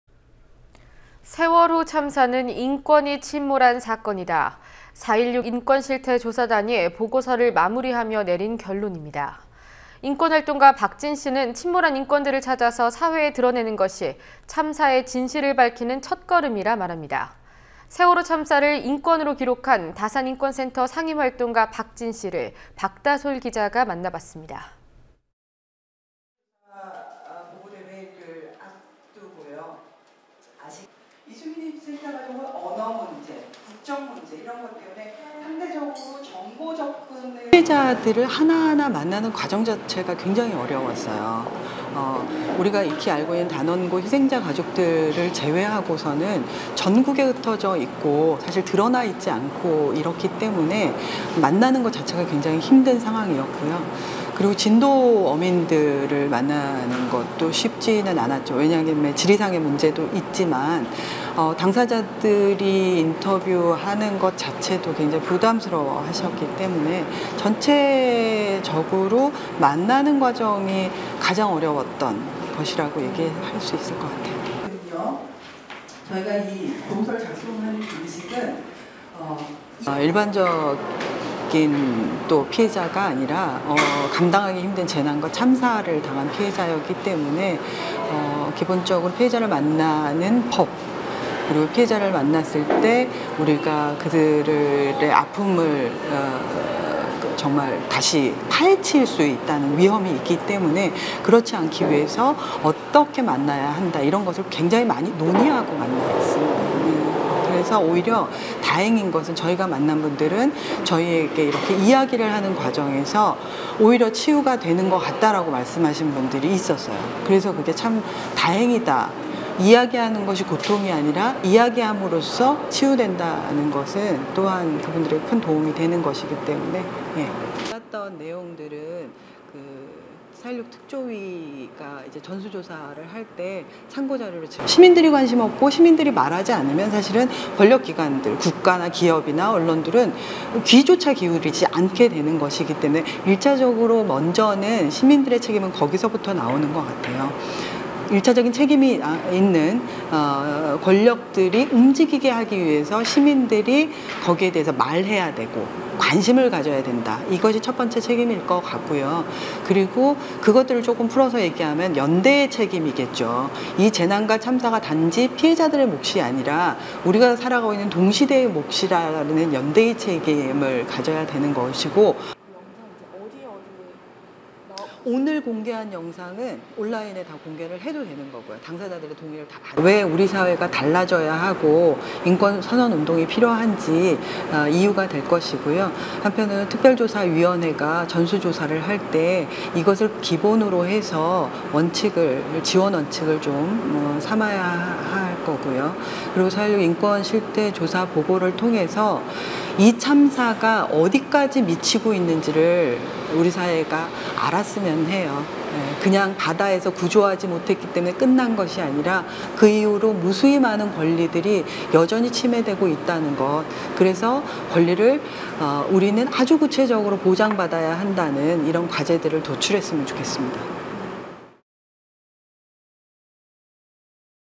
[국민TV] 세월호참사, 인권으로 기록…인권활동가 '박진' (2015.07.15)
[앵커]

‘세월호 참사는 인권이 침몰한 사건이다’416인권실태조사단이 보고서를 마무리하며 내린 결론입니다.

인권활동가 박진 씨는 침몰한 인권들을 찾아 사회에 드러내는 것이 참사의 진실을 밝히는 첫걸음이라 말합니다.

세월호참사를 인권으로 기록한 다산인권센터 상임활동가 박진 씨를 박다솔 기자가 만나봤습니다.